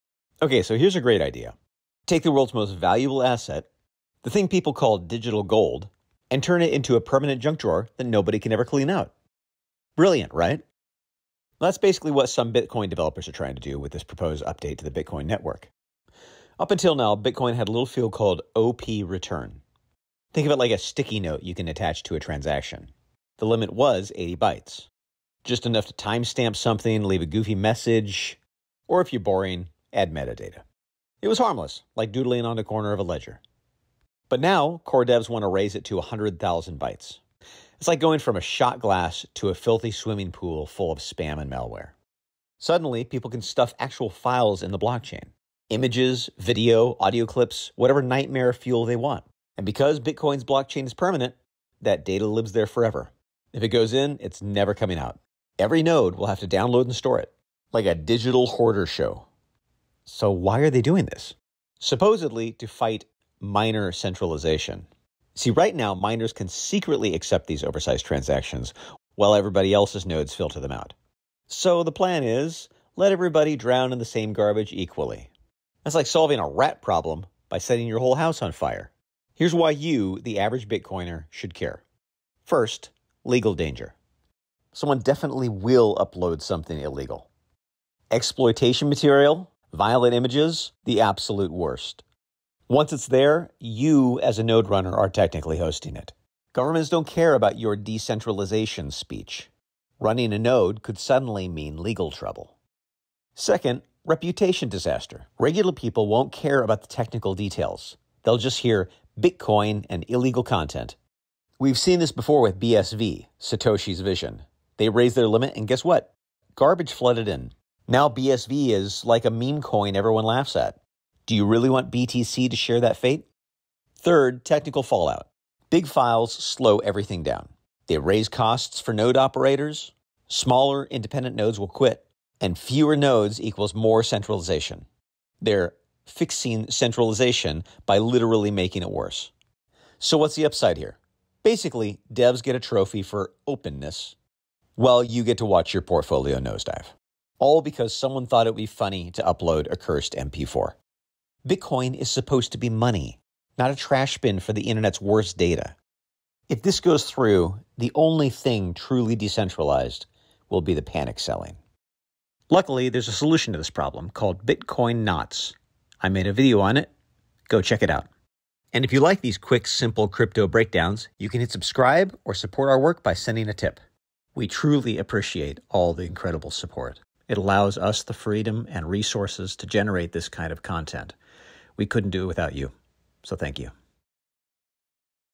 Let's Ruin Bitcoin! - Core V30 and OP_RETURN
Learn how to run Bitcoin Knots to protect your node and keep control over what you relay and store!

Bitcoin Knots is a more configurable version of Bitcoin Core. It gives you filtering tools to block spammy, oversized transactions and avoid being forced to store junk or illegal content forever.

In this video, we break down one of the most controversial Bitcoin updates ever: raising the OP_RETURN limit from a harmless 80 bytes to a massive 100,000 bytes.
Why does that matter? Because this change could turn Bitcoin into a global junk drawer, forcing every node to store files permanently — even illegal ones.

Here’s what we cover:

- Why the OP_RETURN field exists and how it’s being abused.
- The insane jump from 80 bytes to 100,000 bytes.
- The legal nightmare of hosting permanent illicit content on your node.
- Historical example: how BSV became a dumpster fire after making this exact mistake.
- How bloated data weakens decentralization and increases costs for node operators.
- Why developers are claiming this fights miner centralization… and why that’s nonsense.

This update doesn’t just threaten Bitcoin’s reputation.  It could also:
- Scare off new users.
- Drive up costs to run a node.
- Push governments to crack down harder on Bitcoin.
- Lead to fewer independent nodes and more centralization, not less.

If you care about Bitcoin’s long-term survival and don’t want to store someone else’s garbage forever, running Bitcoin Knots is the smartest move.

⚡ Our BOLT12 lightning address is: lno1zrxq8pjw7qjlm68mtp7e3yvxee4y5xrgjhhyf2fxhlphpckrvevh50u0q0fqhu7pzr99f7tcahz202kevm5e45dy8f7htk4q6w43vrnw52yvjqsz2nc3d327u7galh4x44vurd4arslfdy2jd3nug7d9nycx2rxu6dpsqvc7lzacyjgdrqzhmrua6437gdcce8c3wlkqzl0yyqnyaaf8ldrd7f5ereejw4zakn0u259qaglg6lwjpfkhq0s22gezj0mmqj0vcrxlwwtxyn578nht6h26589rzuh3qjr4ah4swqqsgadtey5nd8m8wcva6cxjznu8uv